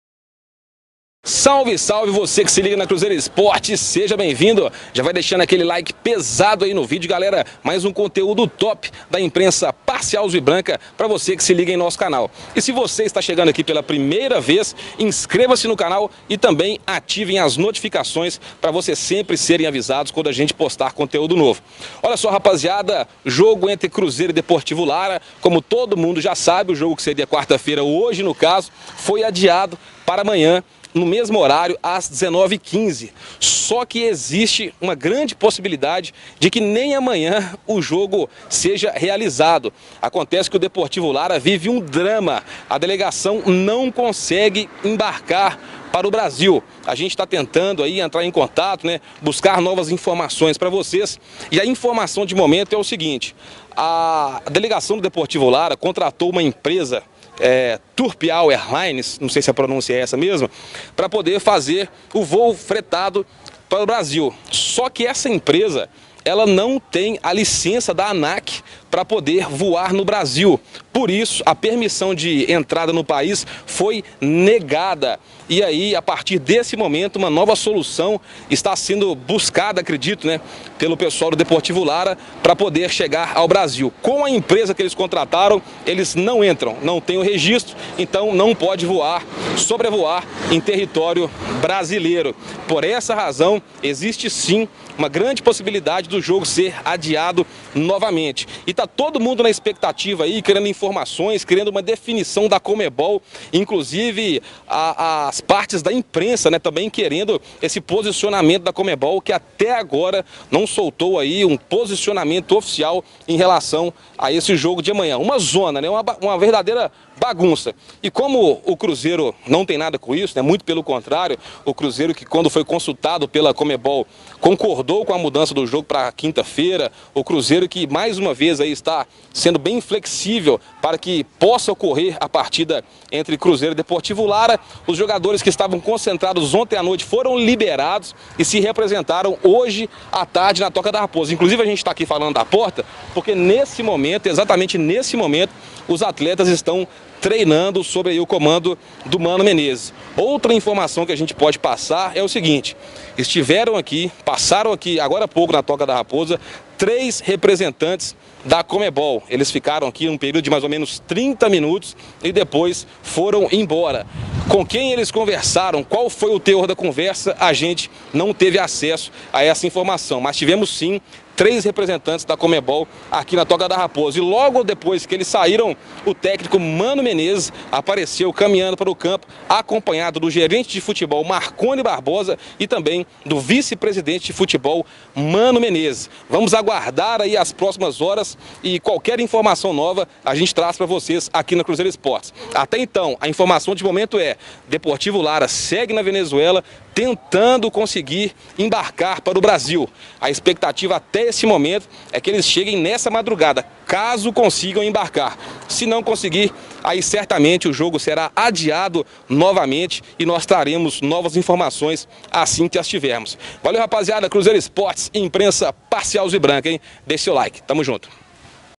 CRUZEIRO X DEPORTIVO LARA ADIADO NOVAMENTE, UMA NOVA DATA PRO JOGO SERÁ ANUNCIADA EM BREVE!
CRUZEIRO SPORTS